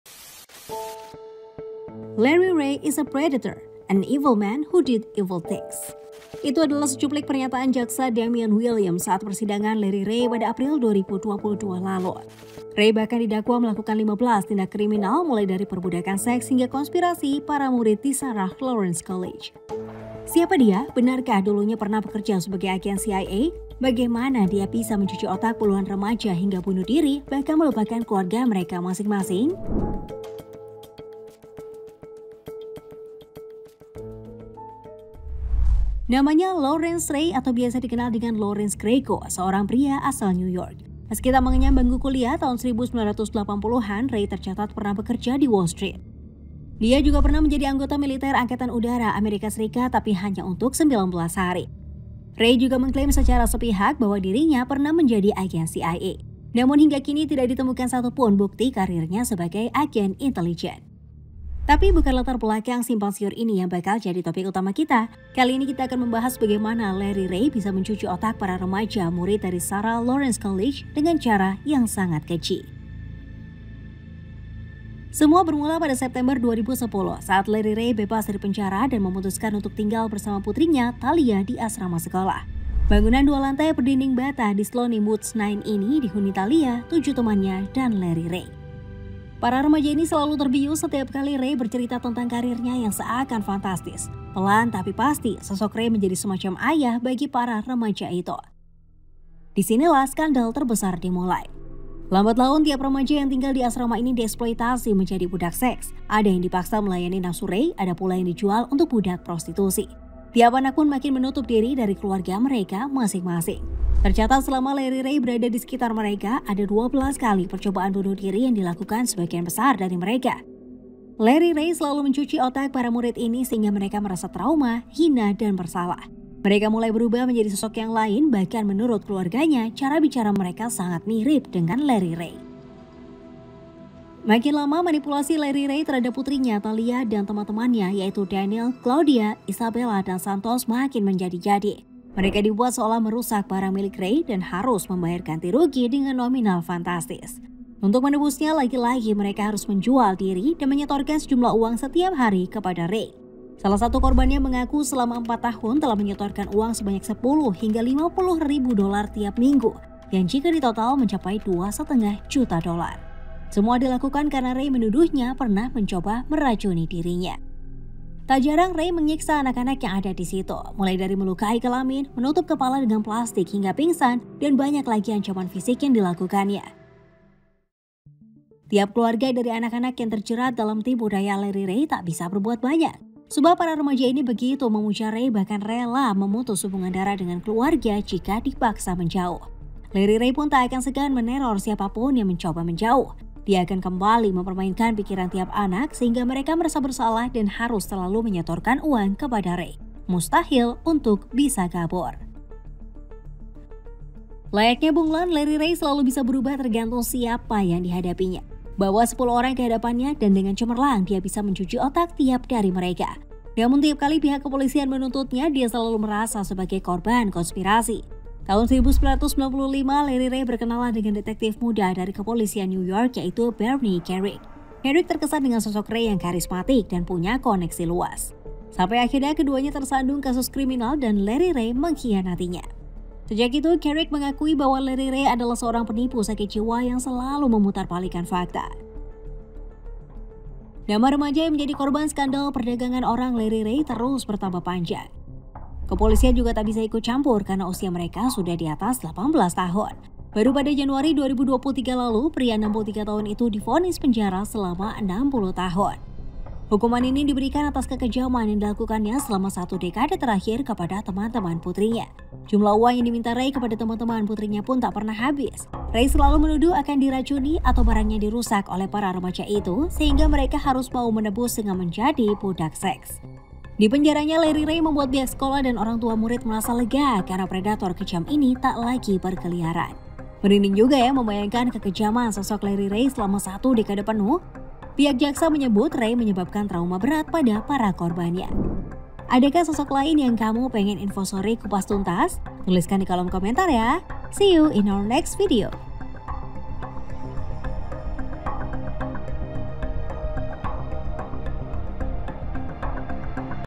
True Crime: Perbudakan Seks Nyata
“Larry Ray is a predator. An evil man who did evil things.”

Itu adalah secuplik pernyataan Jaksa Damian Williams saat persidangan Larry Ray pada April 2022 lalu.

Ray bahkan didakwa melakukan 15 tindak kriminal, mulai dari perbudakan seks hingga konspirasi para murid di Sarah Lawrence College.

Siapa dia? Benarkah dulunya pernah bekerja sebagai agen CIA? Bagaimana dia bisa mencuci otak puluhan remaja hingga bunuh diri bahkan melupakan keluarga mereka masing-masing?

Animasi dibuat oleh InfoSore.

FAQ (Frequently Asked Questions):
A: Tiap hari Rabu dan Sabtu. Tapi kadang-kadang ada video yang butuh waktu pembuatan lebih lama.

Content:
00:28 – Latar belakang yang simpang siur
01:06 – Tinggal bersama di asrama
01:35 – Skandal sekte seks
02:10 – Modus mengancam minta ganti rugi\
02:51 – Cuci otak ala Larry Ray
03:17 – Licik bagai bunglon
03:52 – Vonis 60 tahun penjara
04:32 – Closing